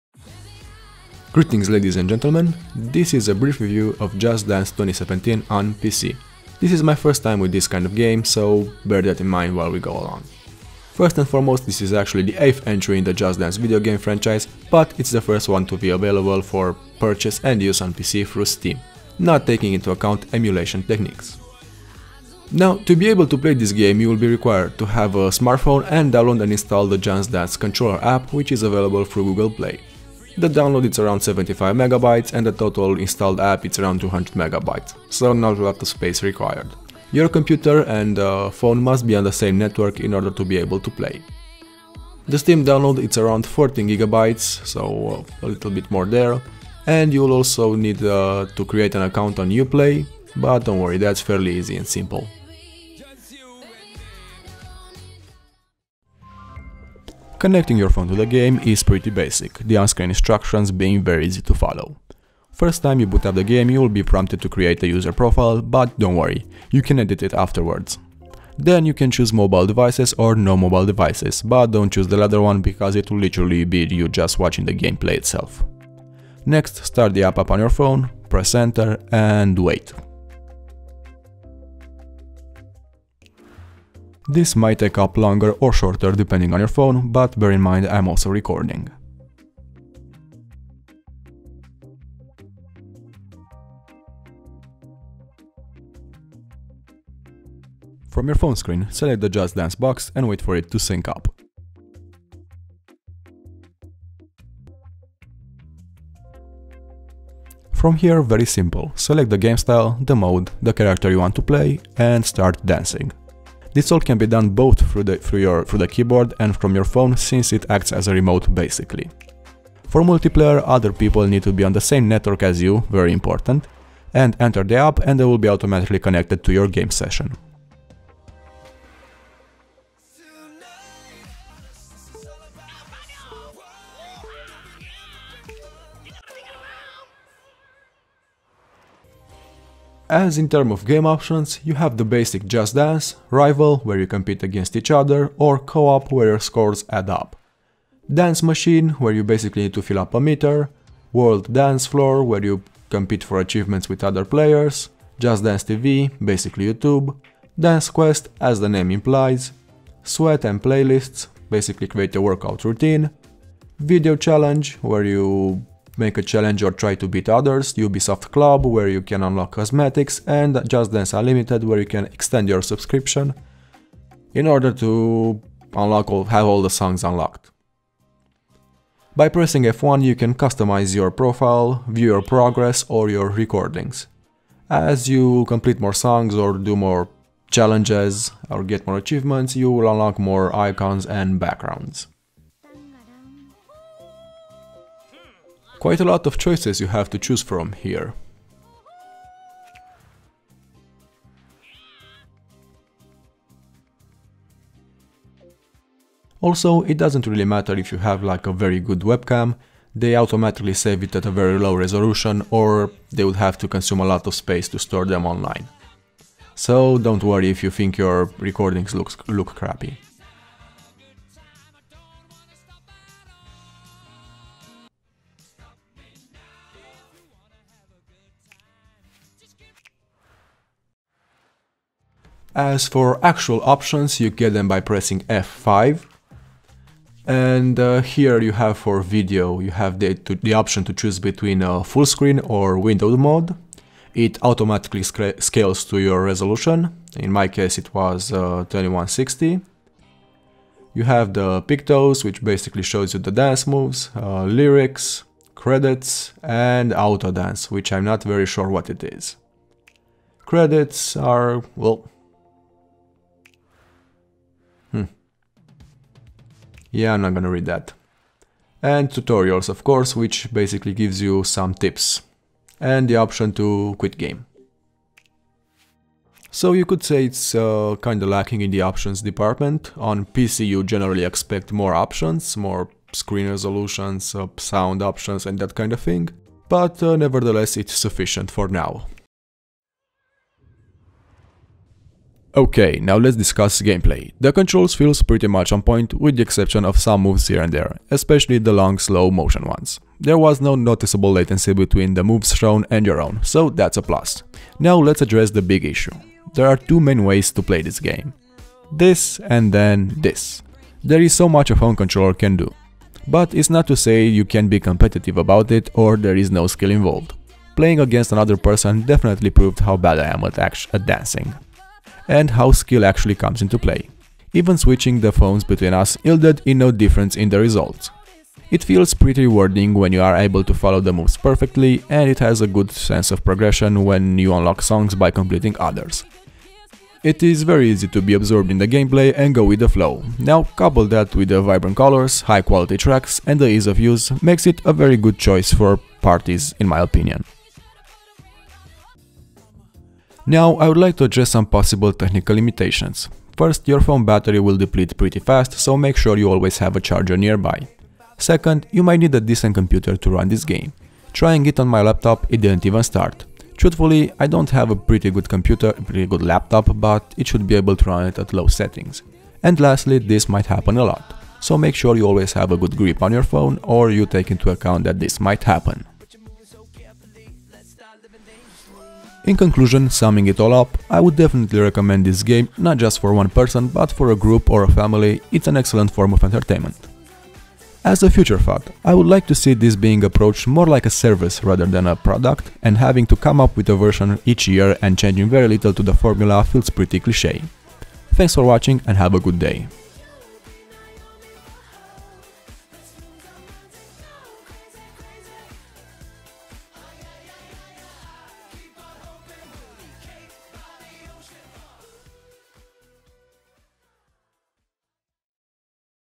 Just Dance 2017 - PC - Review
I always wanted to try out one of this games, and it's pretty fing awesome.